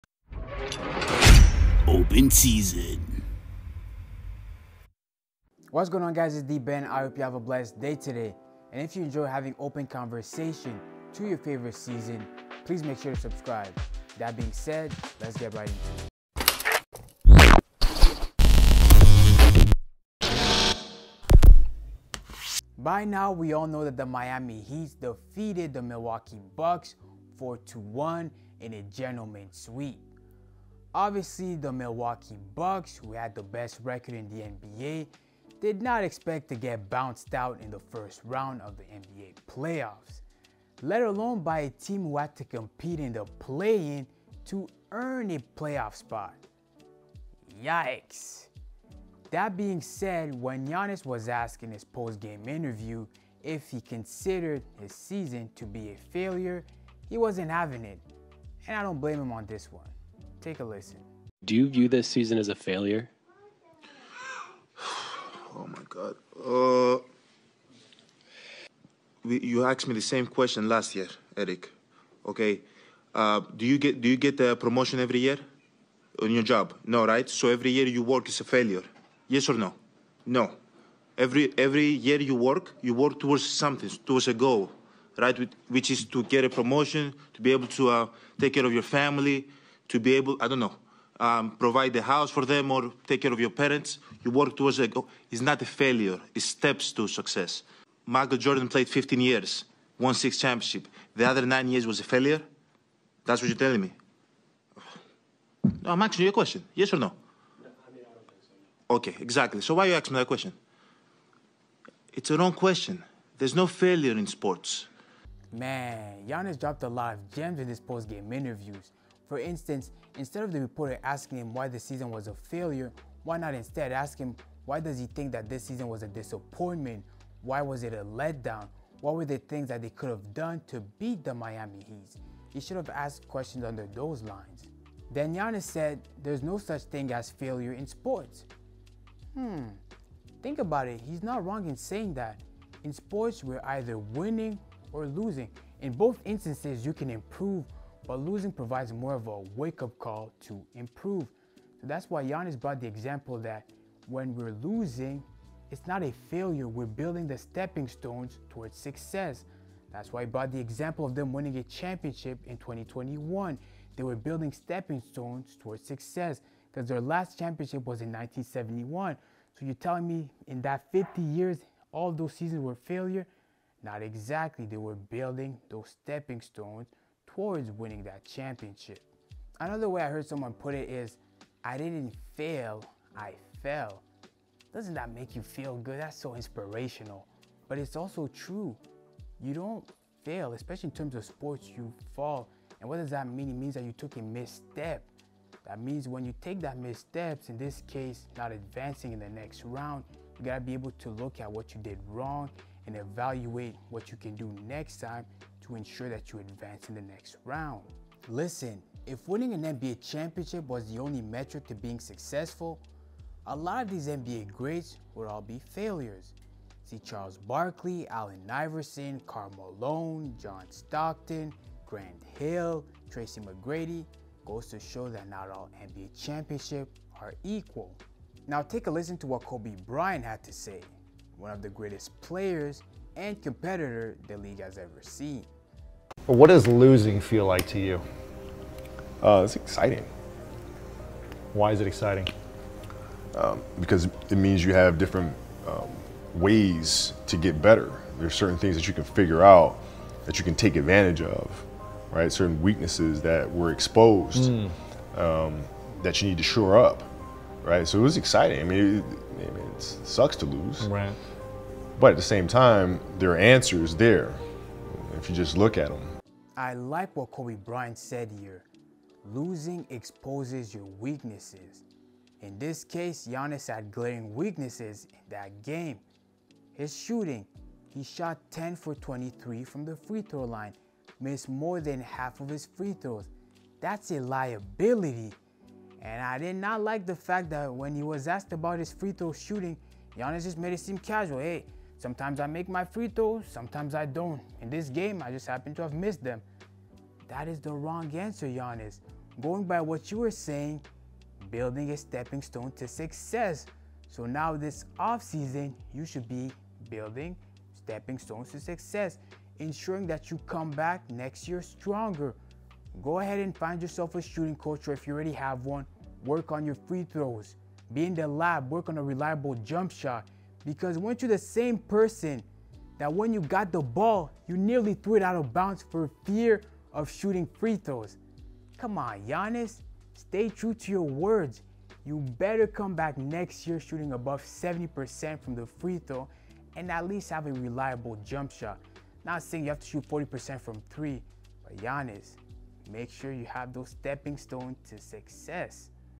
This Is Exactly WHY Giannis Went OFF On This Reporter
In today's special episode we discuss, Giannis Antetokounmpo's postgame comments about failure and why the Milwaukee Bucks season was actually not a failure.

I appreciate you taking the time to listen to the video, and I would love for you to share so that we can grow and improve.